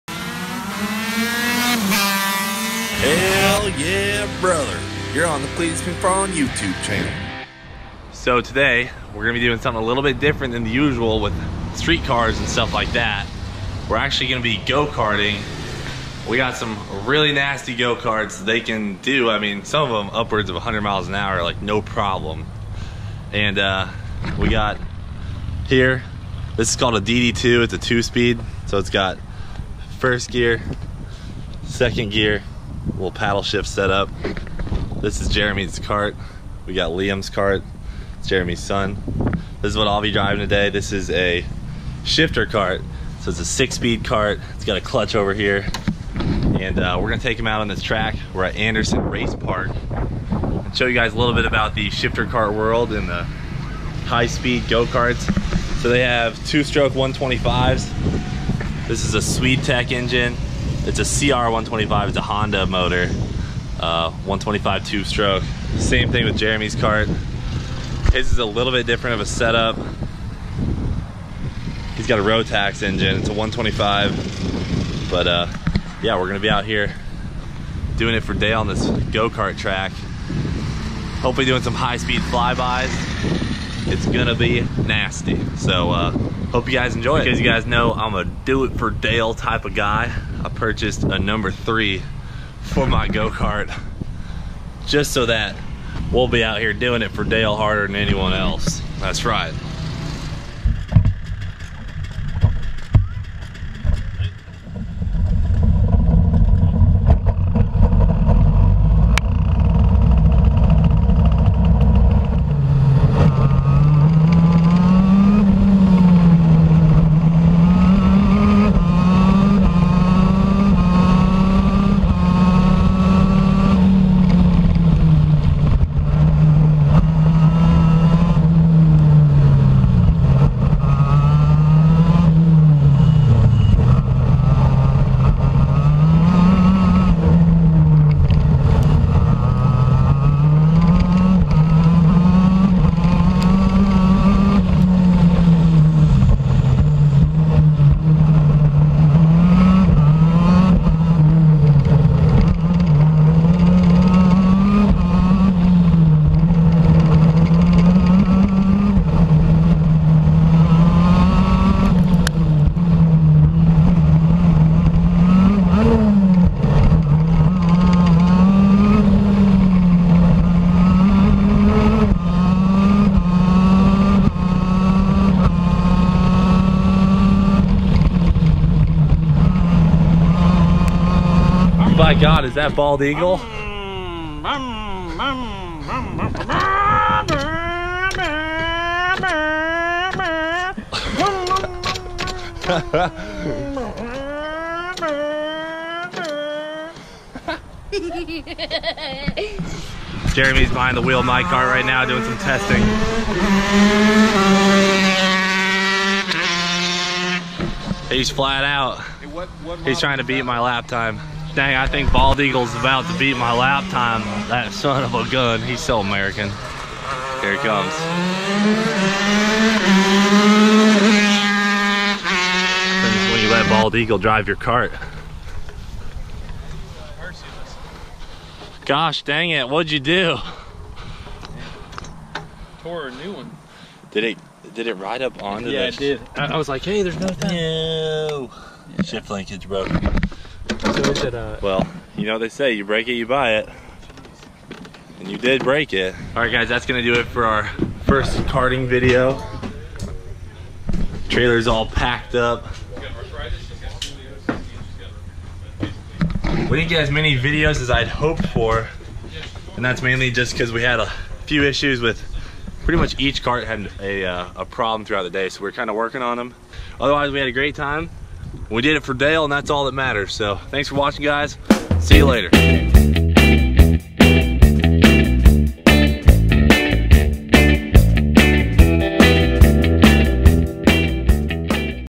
EXTREME Go-Karting on 6-Speed Shifter Karts!
If you've ever had the chance to ride a shifter kart, you know exactly how fun, yet TERRIFYING these things can be! Going 80mph at less than an inch off the ground sure is a blast... We were just out there doin it for DALE! Next, we're taking these bad boys on the streets!